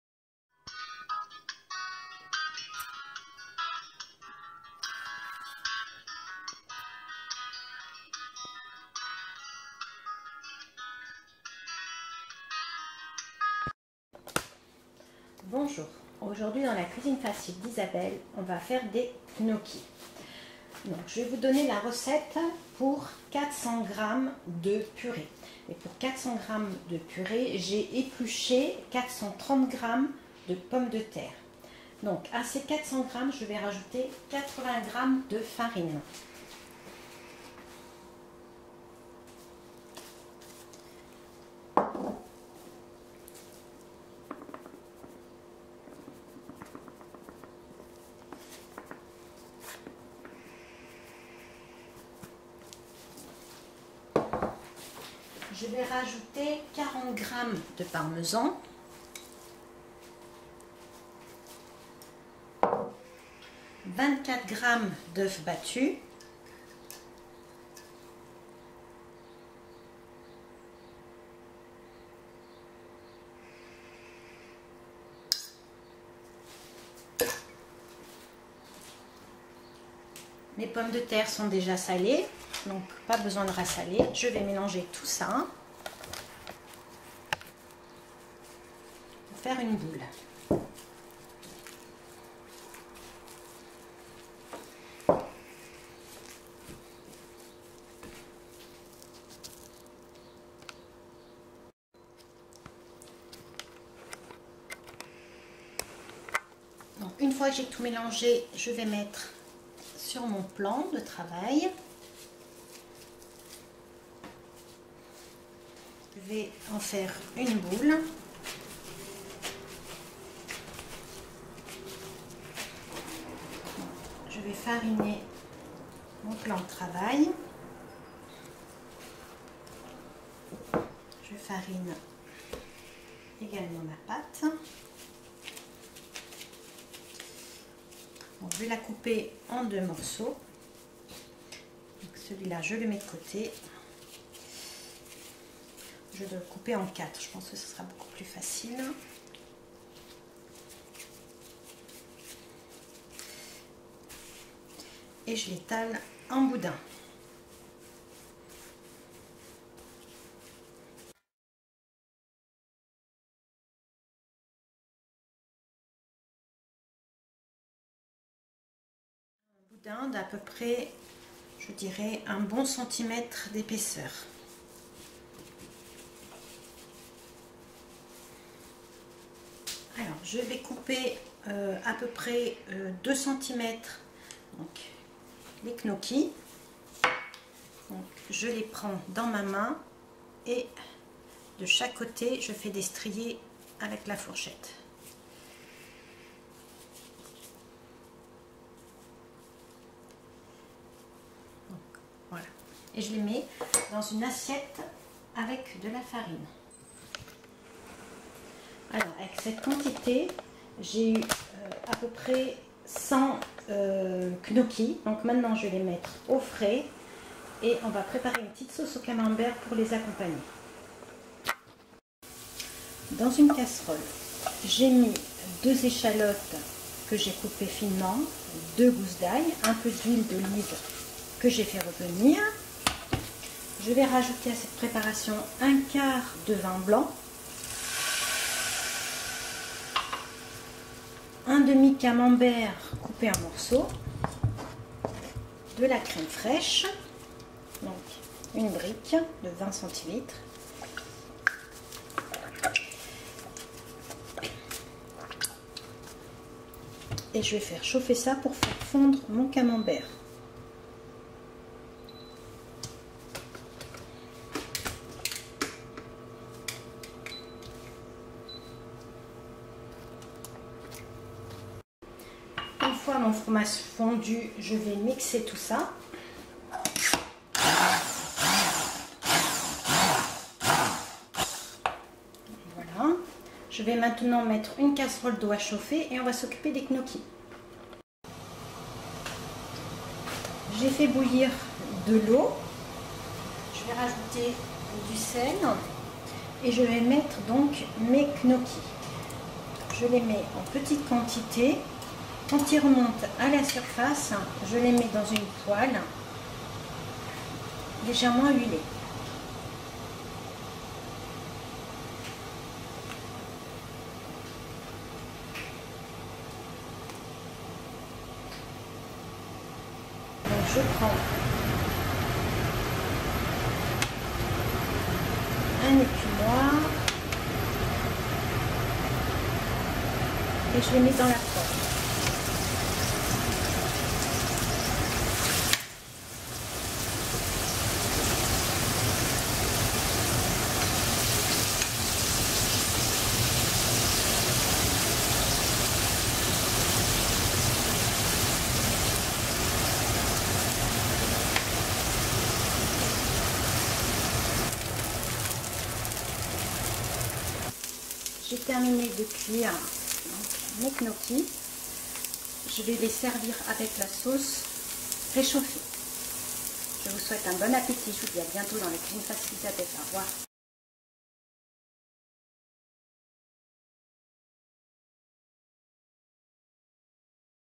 GNOCCHIS MAISON SAUCE CAMEMBERT
200g de purée de pommes de terre
• 50 g de farine
• 20g de parmesan
• 12g d'œuf battus
• Sel ou sel fin
• Huile d’olive
• ½ camembert
• 1 tasse de crème
• Du beurre
• 2 échalotte
• 2 gousses d’ail
• 1/4 verre de calvados
1. Étape 1 :
Dans un saladier, mélanger la purée, la farine, le parmesan, les œufs et saler. Pétrir à la main jusqu'à l'obtention d'une pâte homogène.
2. Étape 2 :
Fariner un plan de travail et former à la main un boudin long et fin
Découper  les gnocchi , faire une boule, avec le dos d'une fourchette  faire des stries
Les déposer sur 1 assiette farinée, les mettre au frai
SAUCE :
Ciseler échalotes et ail, faire revenir dans le beurre, flamber au calvados
Rajouter la crème, le camembert coupé en morceau, une fois fondu, mixer la crème, la réserver

Faire bouillir de l'eau salée. Mettre les gnocchi dans l'eau bouillante, une fois remonter à la surface les récupérer à l’écumoire, les mettre dans une poêle bien chaude ,les faire griller sur chaque face
servir avec la sauce